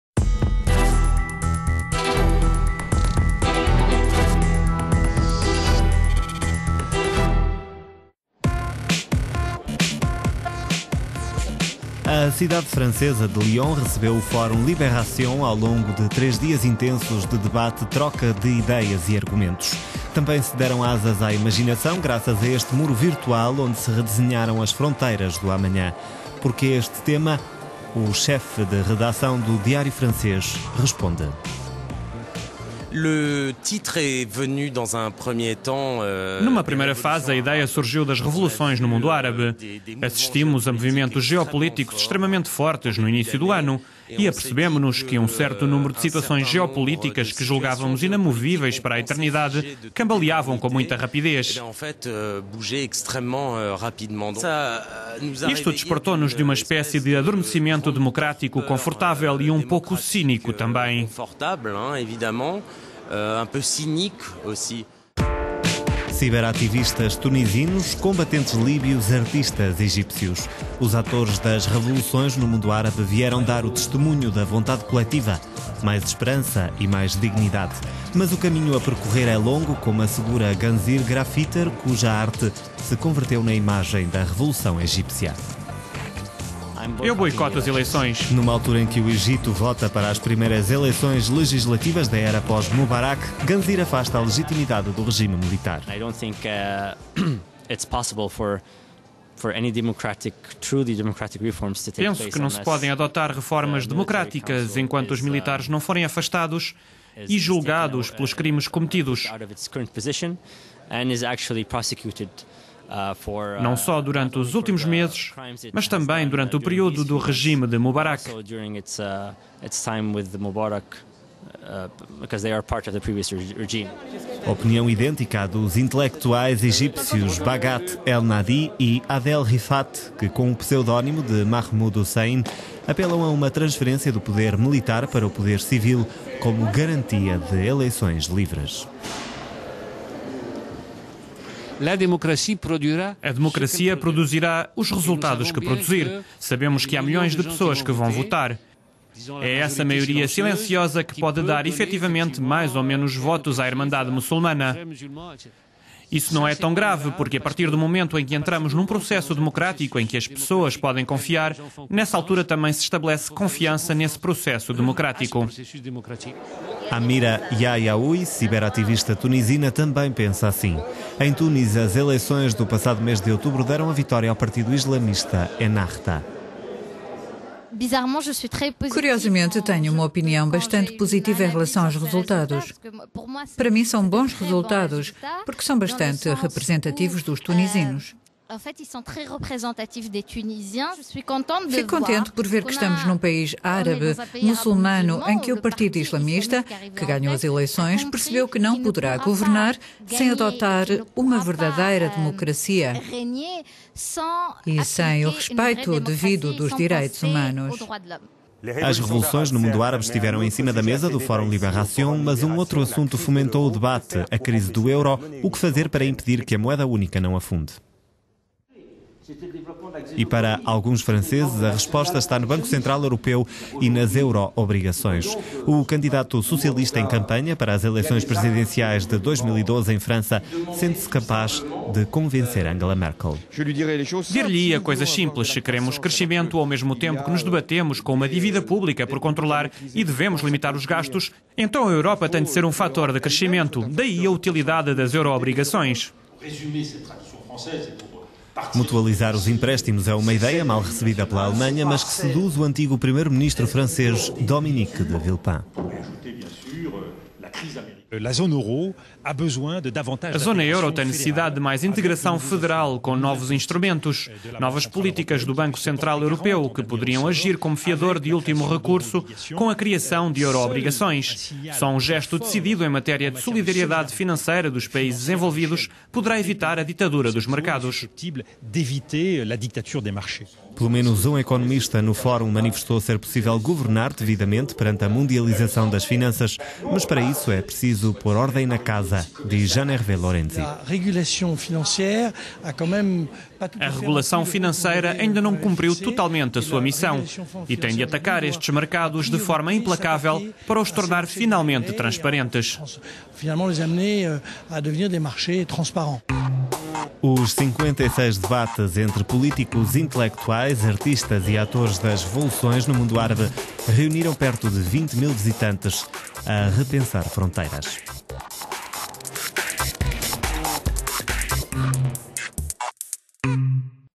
euronews focus - Primavera árabe e crise do euro em destaque no Fórum "Libération"
A cidade francesa de Lyon recebeu o Fórum "Libération" ao longo de três dias intensos de debate, troca de ideias e argumentos.